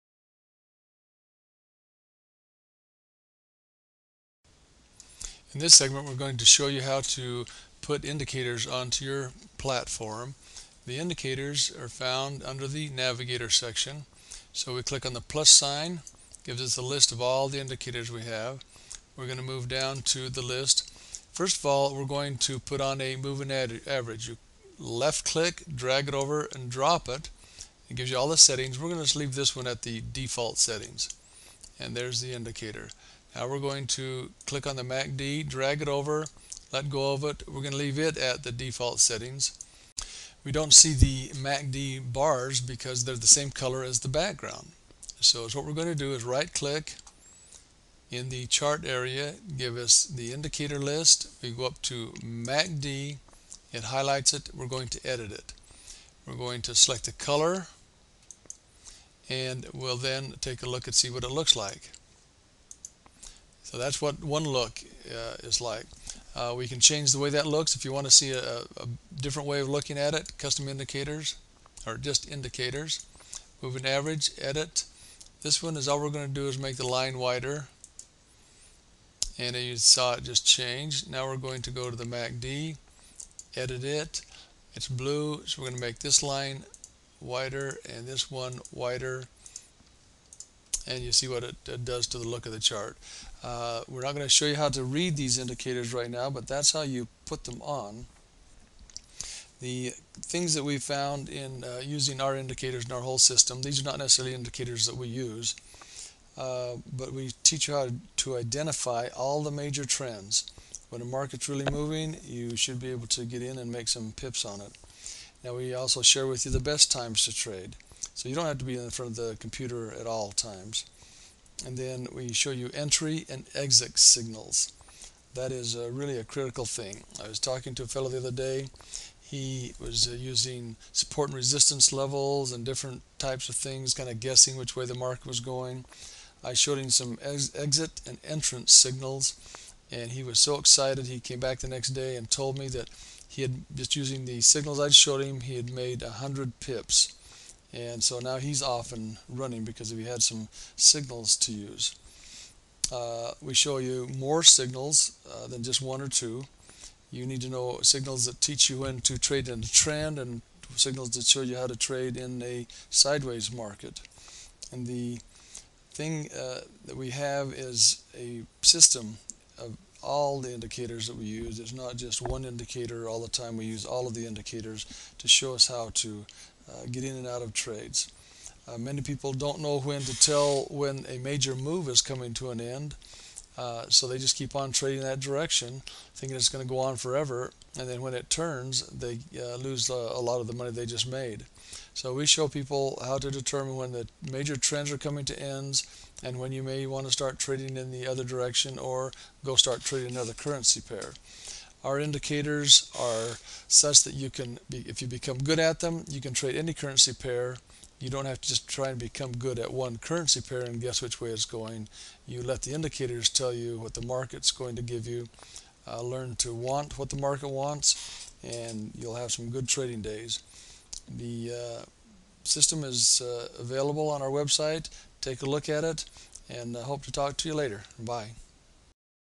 Forex MetaTrader 4 MT4 Platform Part 13 - Forex Indicators - Forex Education - Learn Forex
Watch how to apply indicators to the Meta Trader 4 trading platform
Learn how to put indicators on the charts and customize the settings on the Forex MetaTrader 4 MT4 platform.